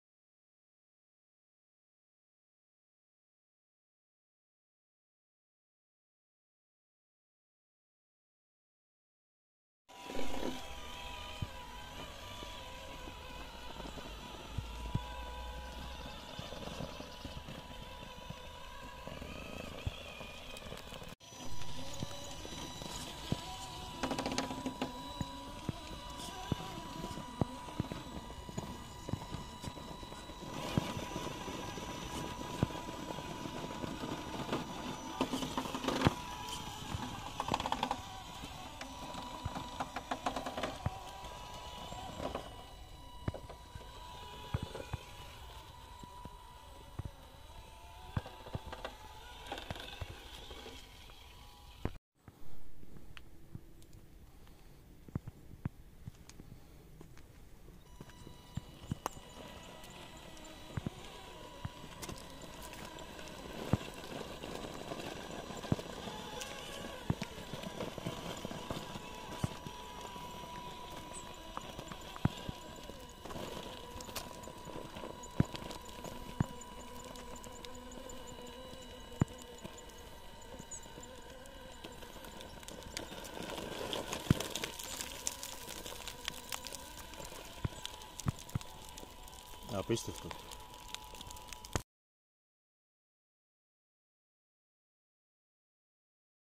TRAXXAS TRX-4 with Proline Ford Raptor body.
RC Team Thessaloniki.
The body it needs a little more cut.